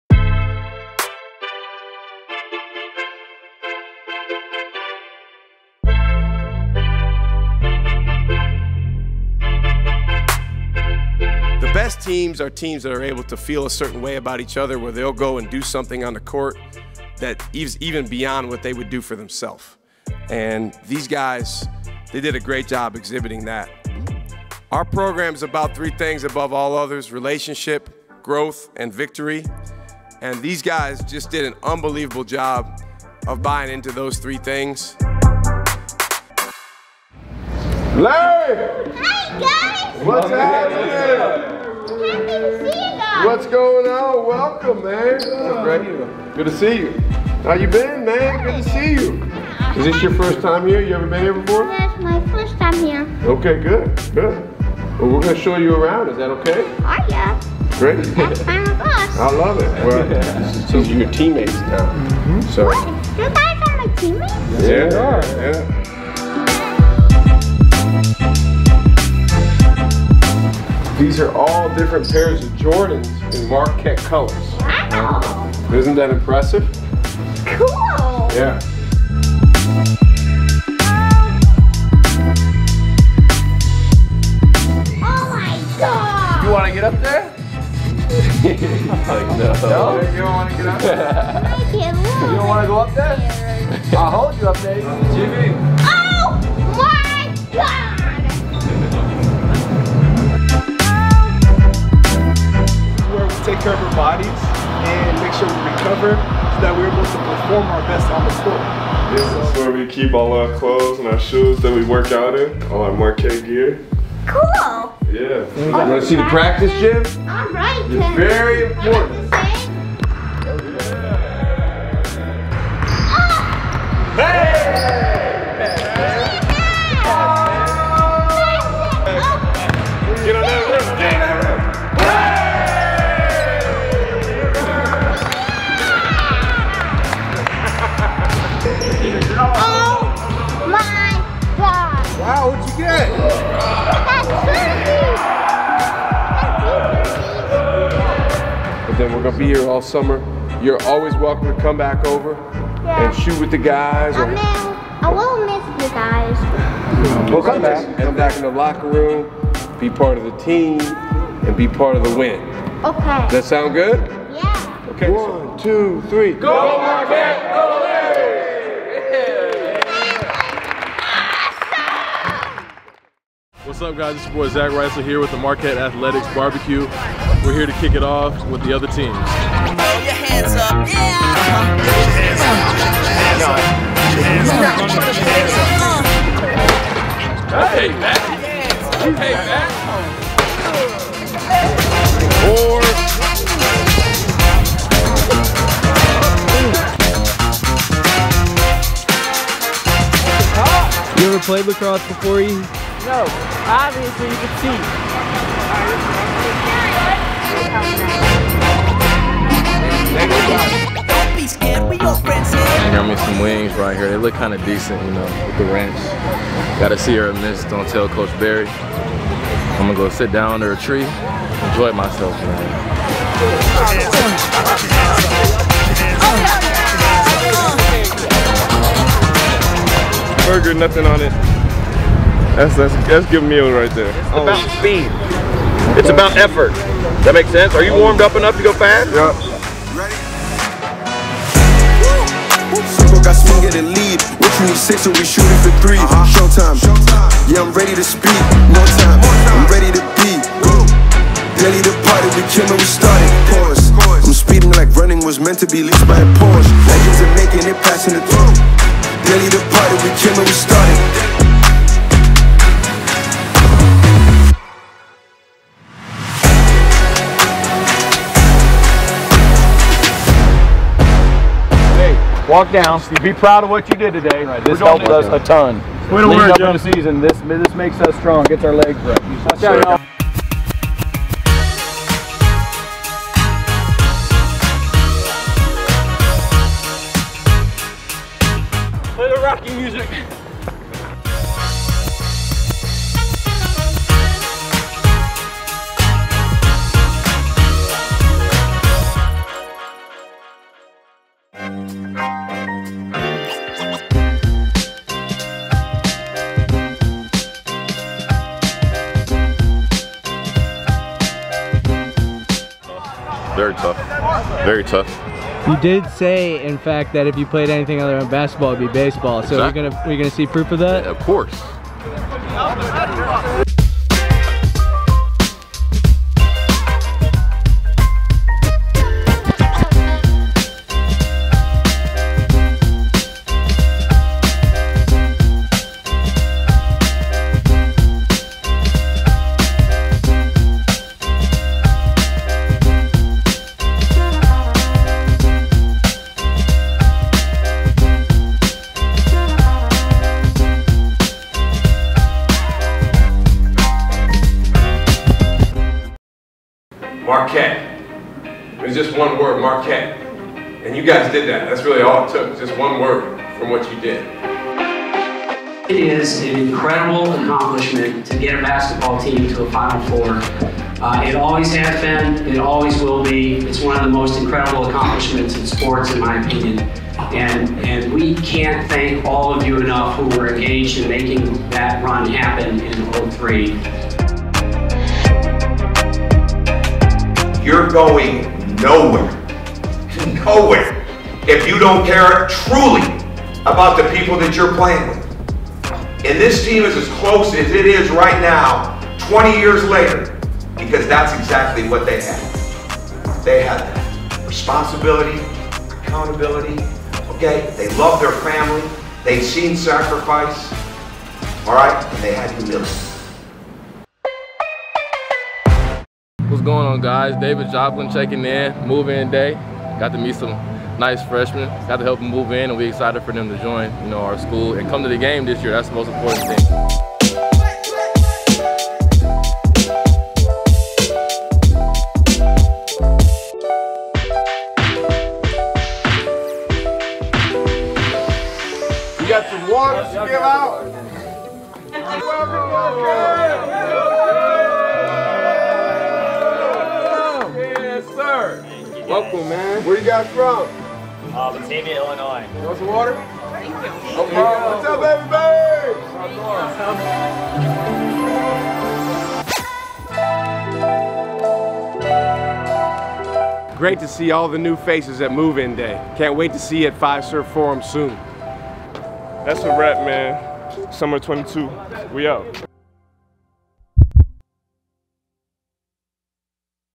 whoWeAre | Episode 12: Summer 2022 Recap
In this episode of whoWeAre presented by Gruber Law Offices, we look back at all of the events that happened over the course of our offseason. From our end-of-year banquet, to summer workouts, this episode has it all!